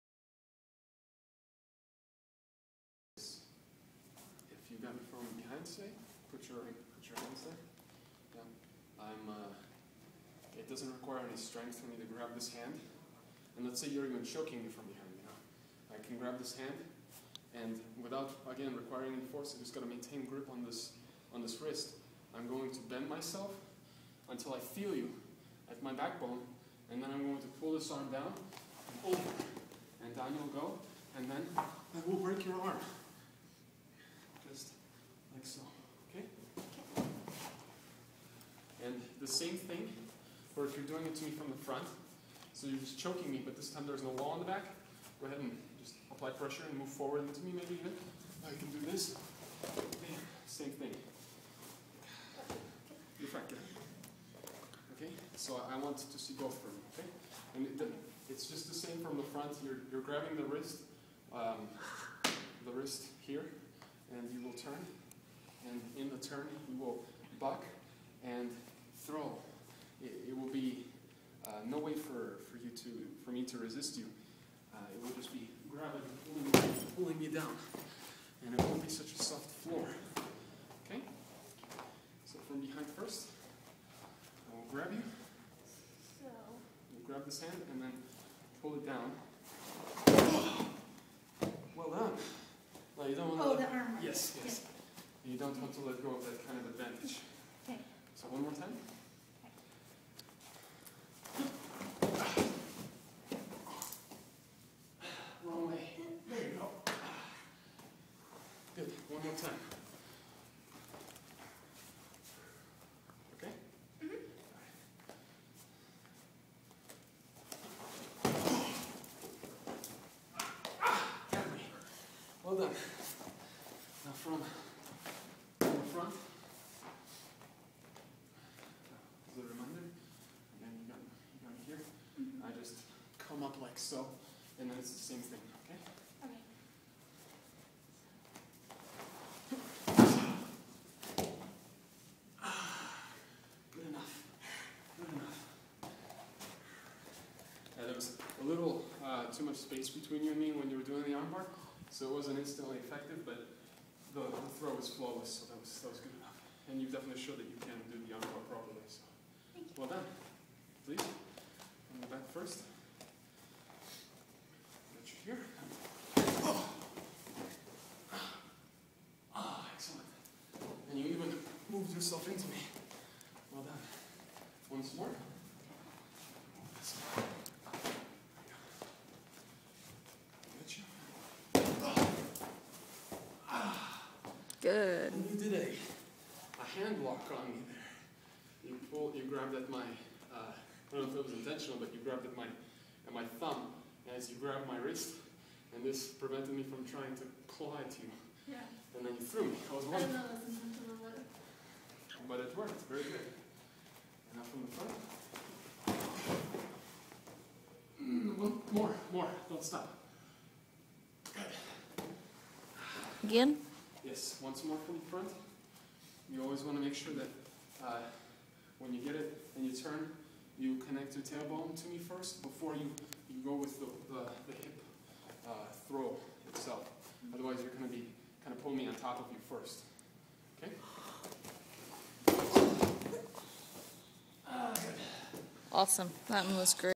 Hip Throw Attacks Front and Back
Defense for attacks from behind and front utilizing hip-throws.